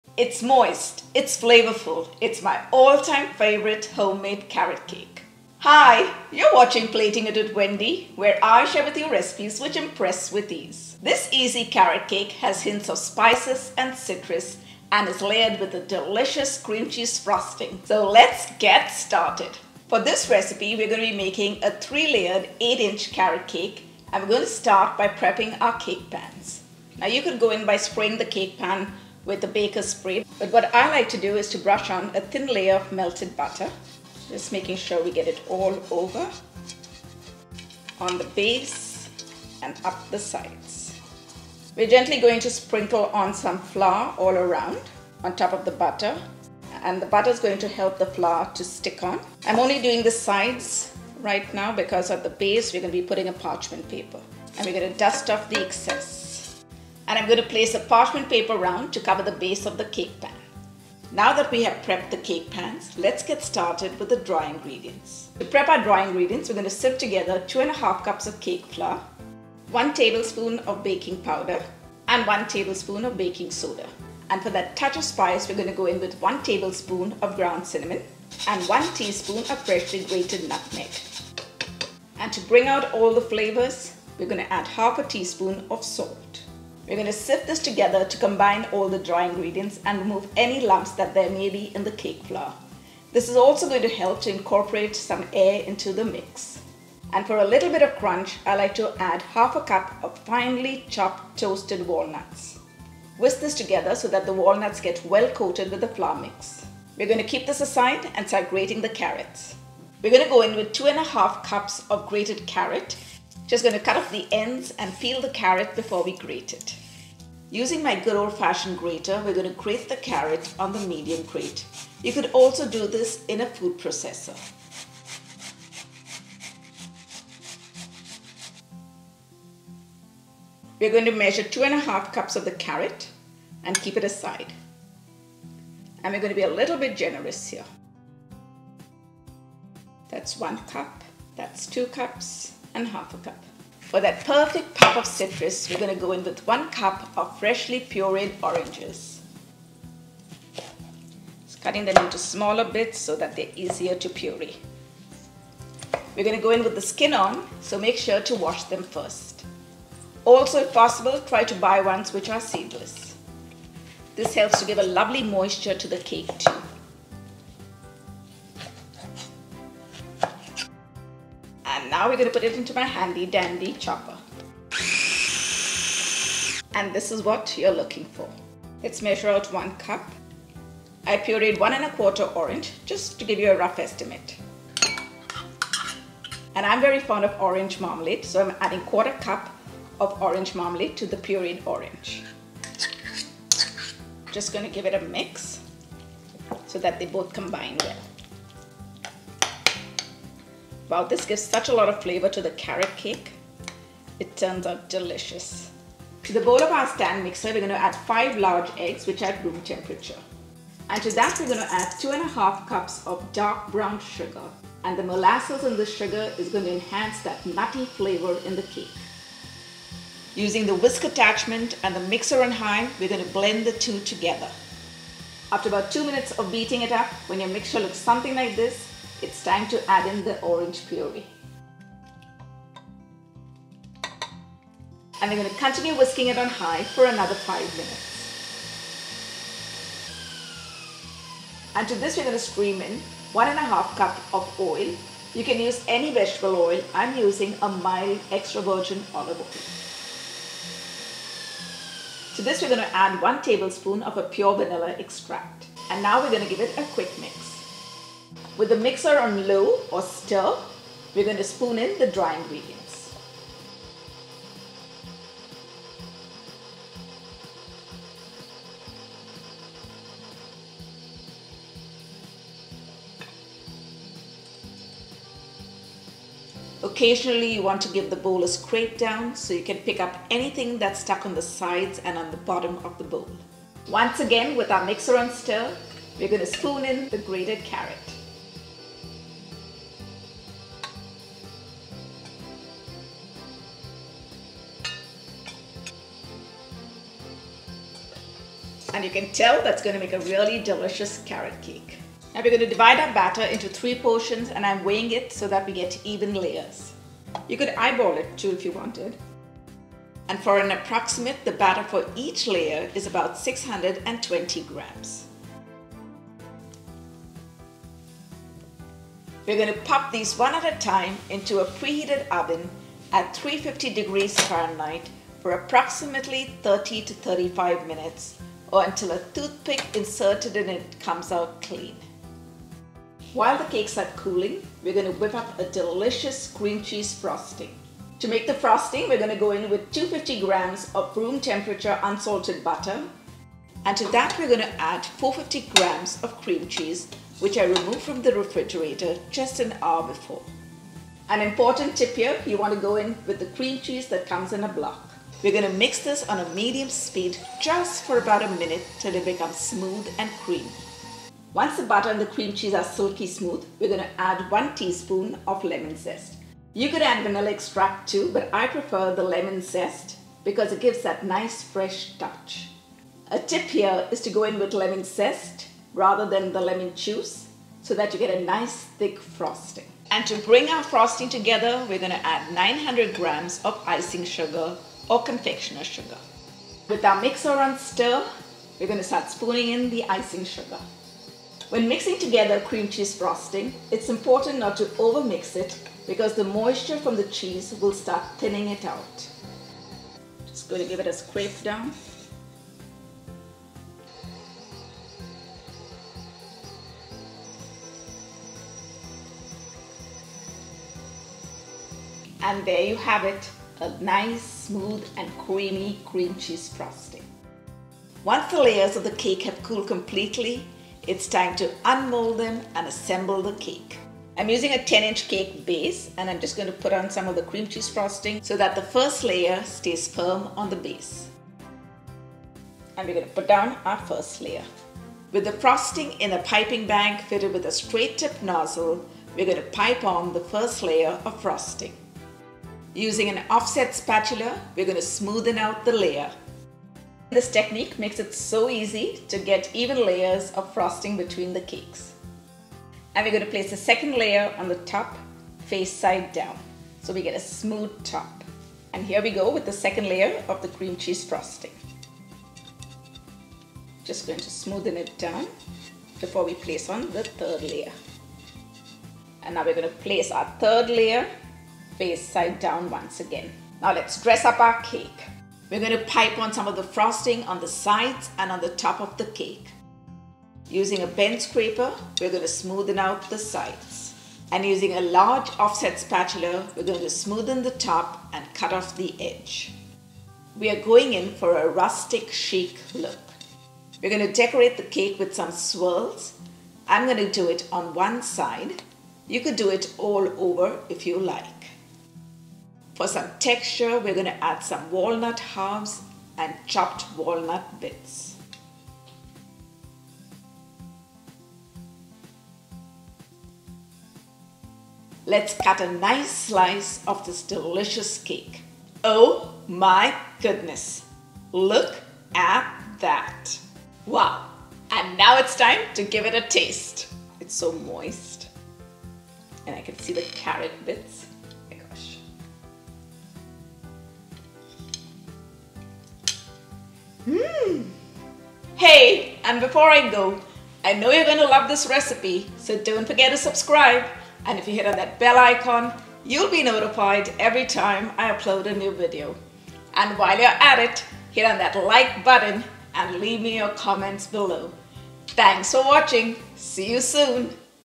Super MOIST Carrot Cake with Cream Cheese Frosting | Baked Favourites
It's moist, it's fluffy, it's my all-time favourite homemade Carrot Cake. This easy Carrot Cake has hints of spices and citrus, and is layered with a delicious cream cheese frosting! I hope you enjoy this classic spring time dessert!